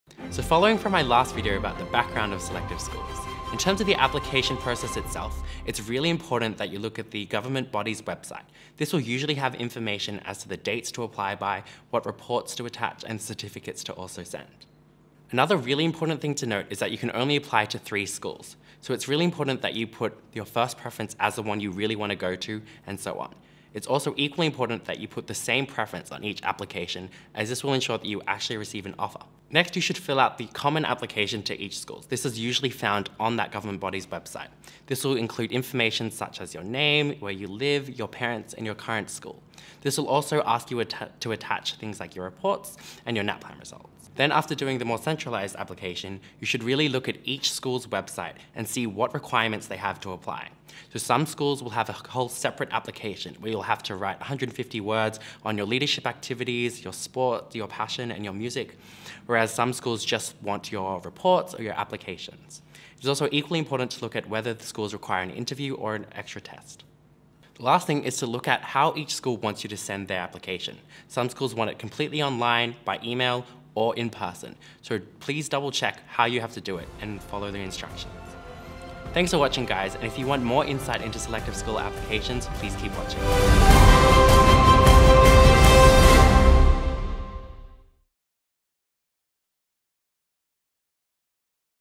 VCE Selective Schools - Application Process
Revision videos for selective school entry. This series includes:

- Application process for selective schools
- Numeracy skills
- Writing skills
- Interview tips for selective schools